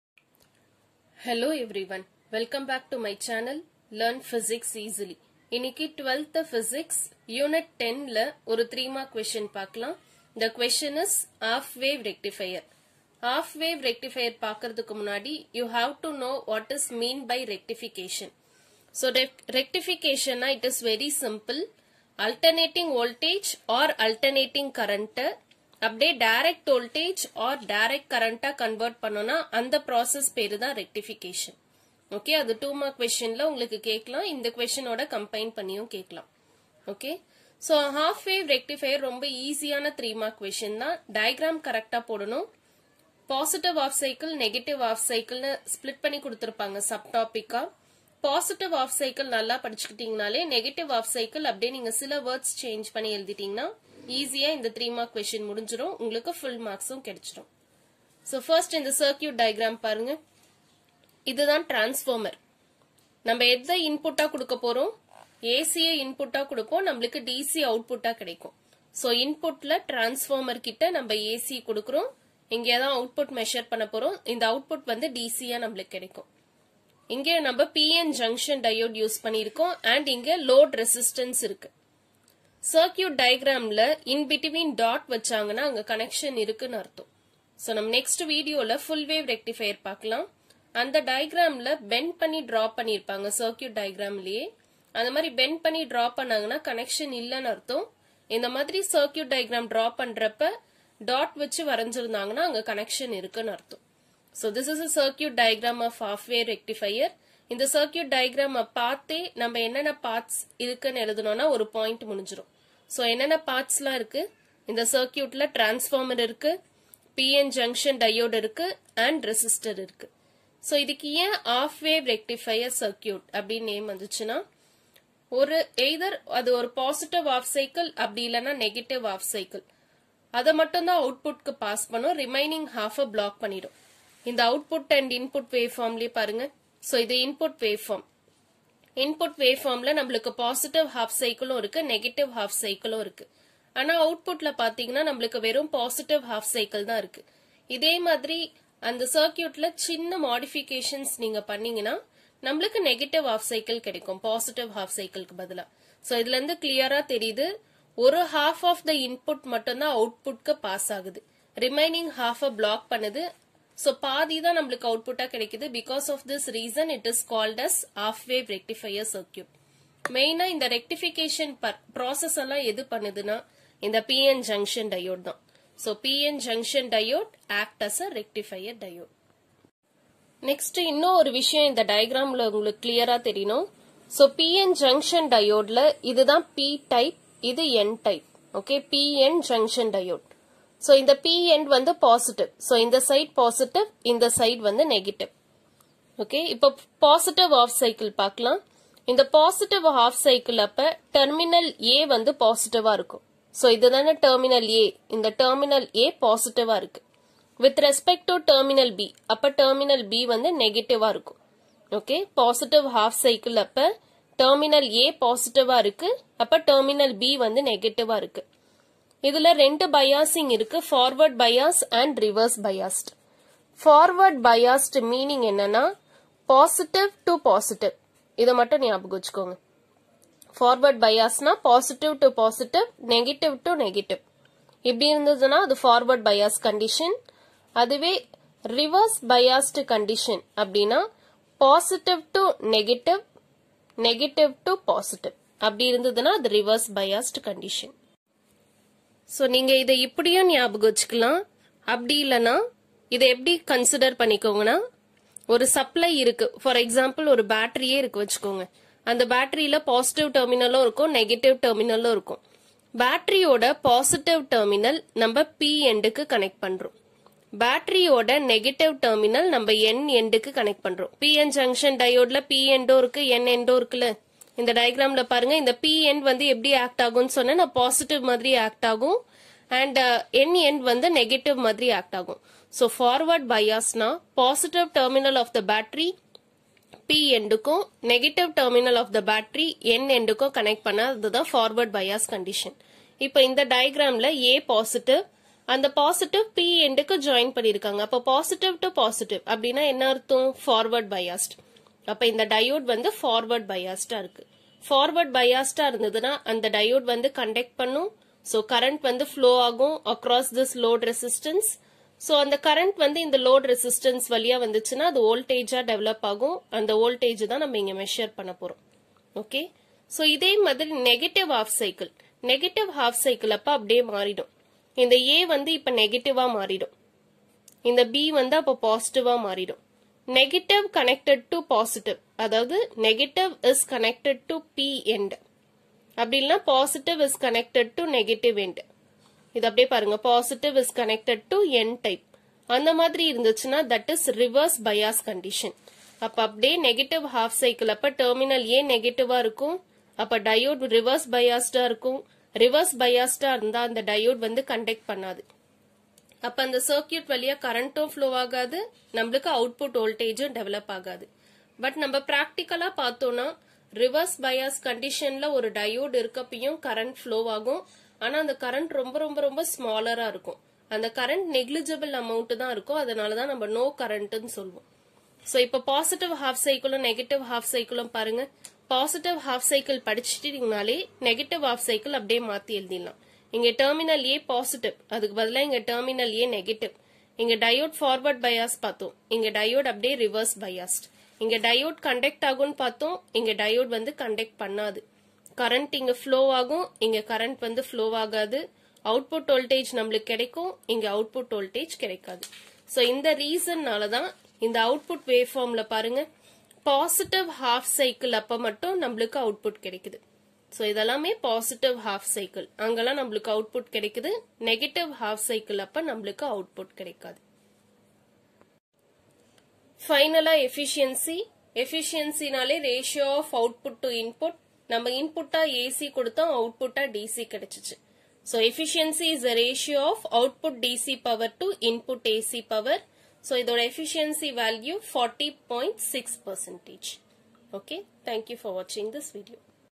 Half wave rectifier | Unit 10 Electronics & Communication | 12 Physics Samacheer kalvi.
Unit 10 Electronics and communication
12 Physics Samacheer kalvi.
Half wave rectifier - 3 mark.
Very easy question.
Just study positive half cycle and negative half cycle is exactly the opposite.
Efficiency of half wave rectifier is 40.6%